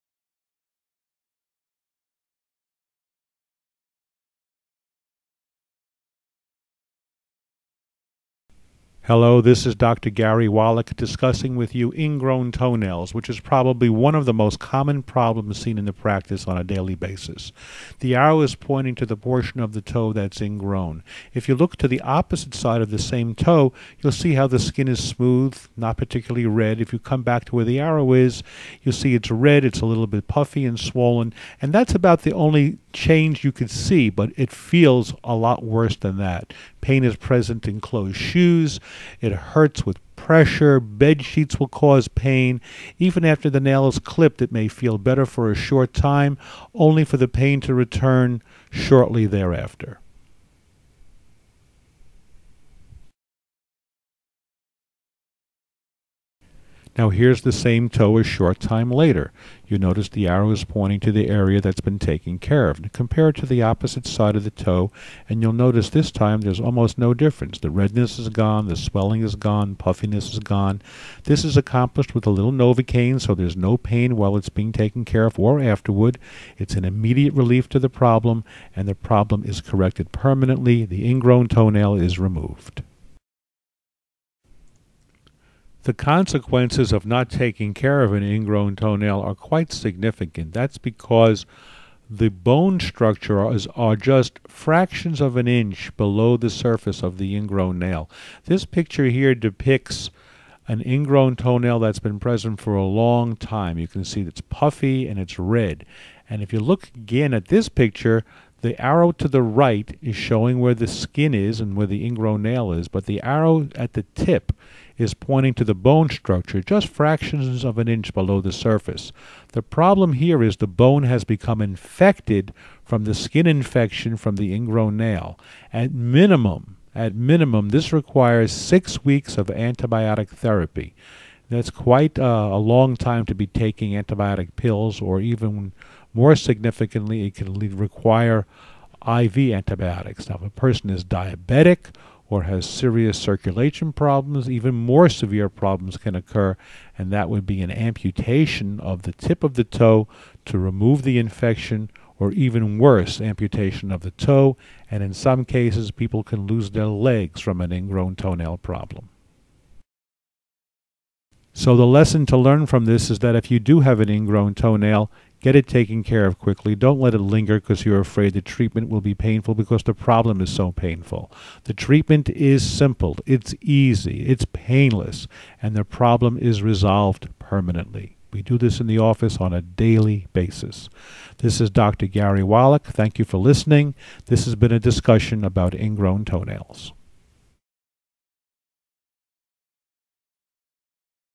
Treatment for Ingrown Toenails
Find out about some helpful treatments for ingrown toenails at Coral Ridge Podiatry!